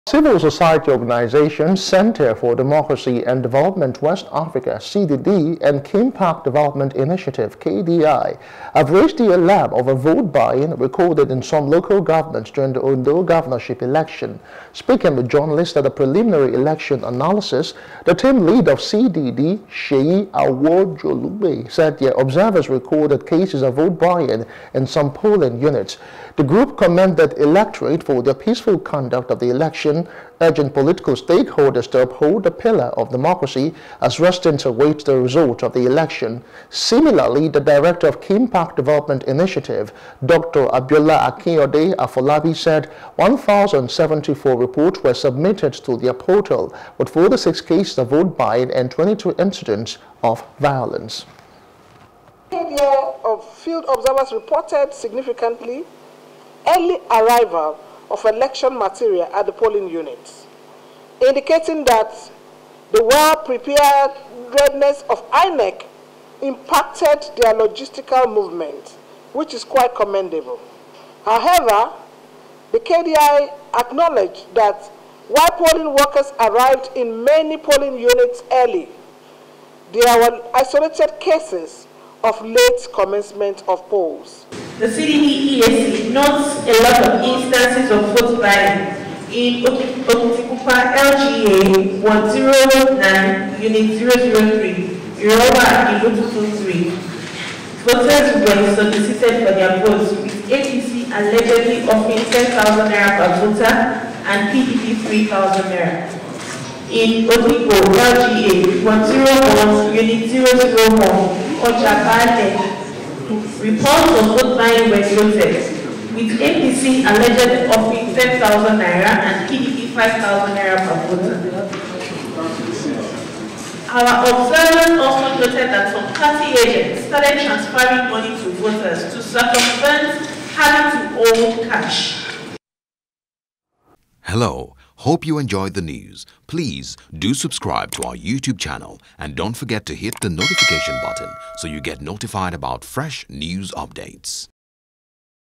Ondo Election: Groups Laud D.S.S. For Arresting Suspected Vote Traders
Civil Society Organizations, Centre for Democracy and Development West Africa CDD and Kimpact Development Initiative KDI have raised the alarm over vote buying recorded in some local governments during the Ondo gubernatorial election.
Speaking with journalists at the preliminary election analysis, the team lead of CDD, Seyi Awojolugbe said their observers recorded cases of vote buying in some polling units.
The group commended electorates for their peaceful conduct of the election, urging political stakeholders to uphold the pillar of democracy as residents await the results of the elections.
Similarly, the Director of Kimpact Development Initiative KDI, Dr Abiola Akinyode Afolabi said 1074 reports were submitted to their portal with 46 cases of vote buying and 22 incidents of violence.